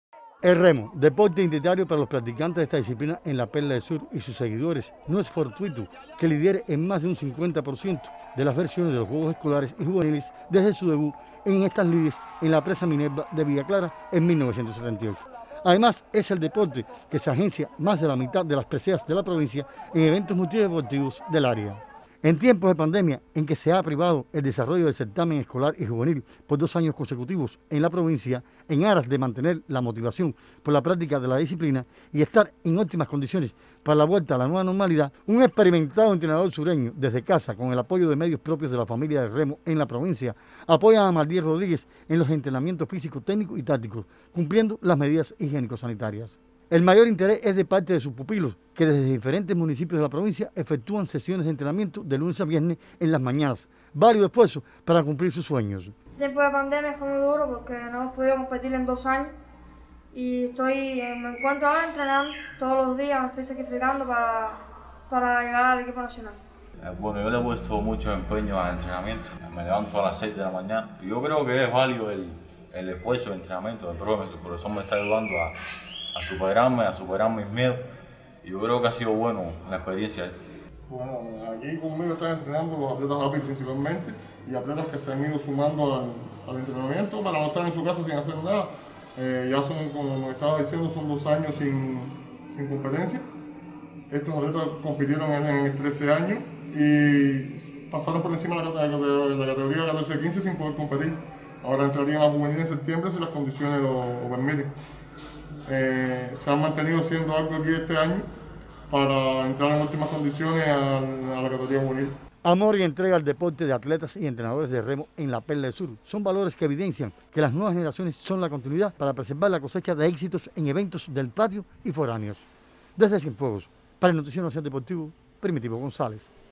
Ingenio ante el coronavirus.
Como consecuencia de la pandemia por segunda ocasión nos privamos de los Juegos Escolares y Juveniles, no obstante el experimentado entrenador Magdiel Rodríguez Santana se las ingenia para mantener la motivación entre sus pupilos.